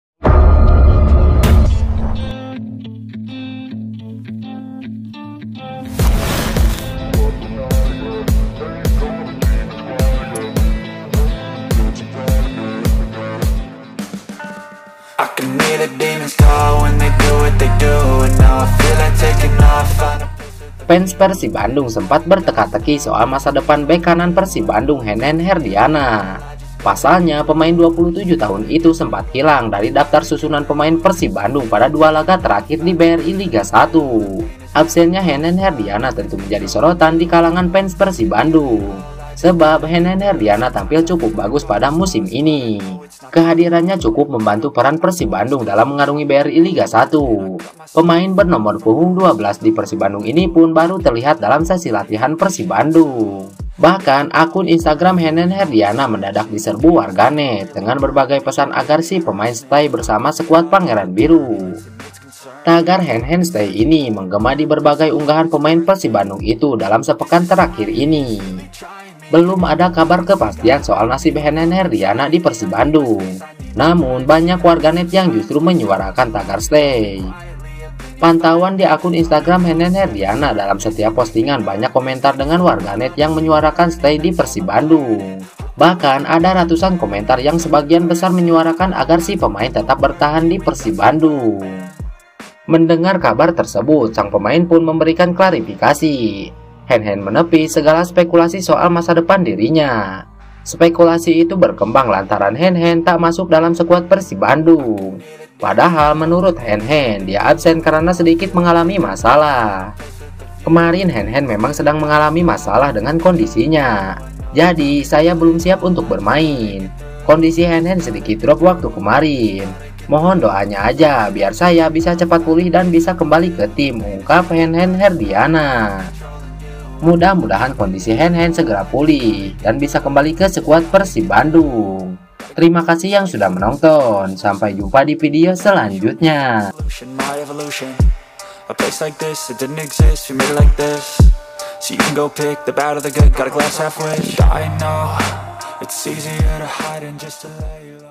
TERJAWAB SUDAH😭Klarifikasi Lord Henhen Usai Namannya Masuk Daftar Pemain Yang Hengkang!
👉Thanks to mation Backgrounds :
Thanks to
 Sumber Narasi Disadur Dari Artikel :